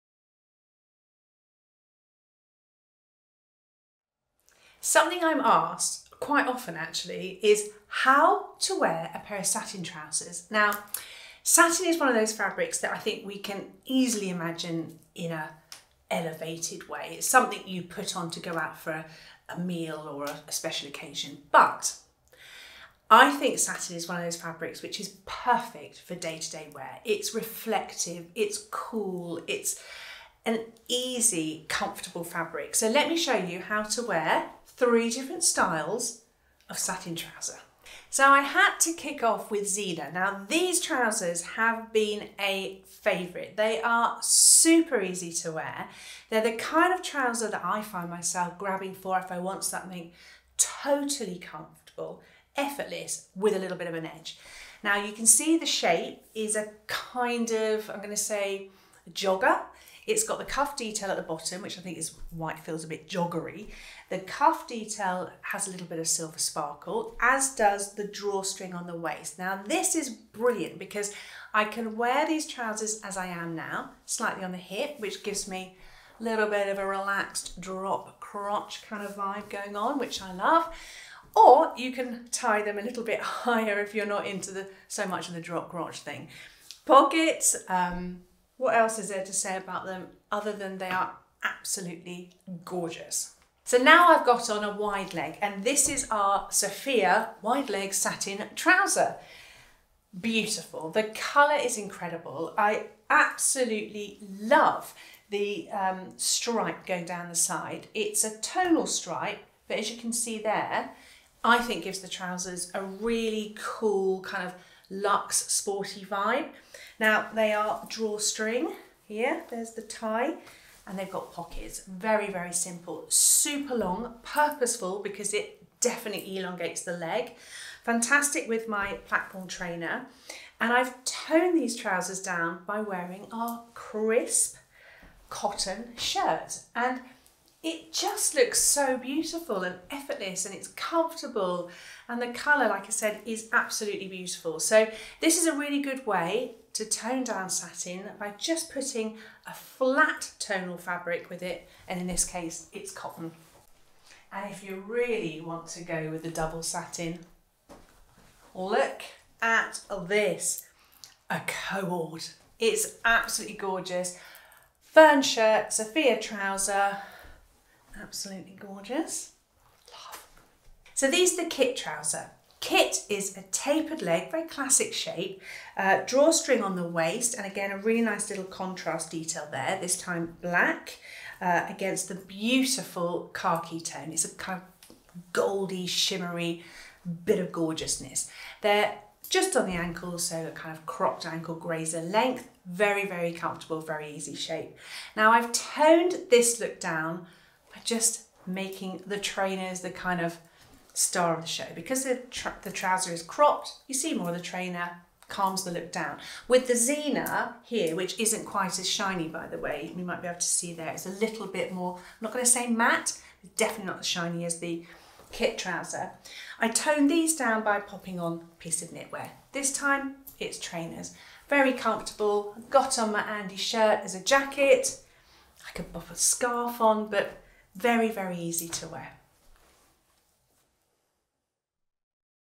Style Guide | How To Wear Satin Trousers
A satin trouser is a tried-and-tested favourite in the Busby & Fox collection.
By combining effortlessly lustrous fabric with clever cuts and details that provide practicality and comfort, we've created a range of trousers that are versatile, eye-catching and seamlessly slot into your workday and weekend wardrobes.

In our latest styling guide, Emma shines a spotlight on these standout styles, sharing her tips on how to wear satin trousers to create a functional yet polished look.

🛍️  THE LOOKS 🛍️

0:38: The Zena Satin Sparkly Cuff Trouser, paired with the Candice Top & Dina Chunky Knit Cardigan
1:33: The Sophia Wide Leg Satin Trouser, paired with the Florrie Shirt
2:33: The Sophia Wide Leg Satin Trouser, paired with matching Fern Satin Shirt
2:50: The Kit Satin Trouser, paired with the Celia Crepe V-Neck Top & Andi Crepe Shirt.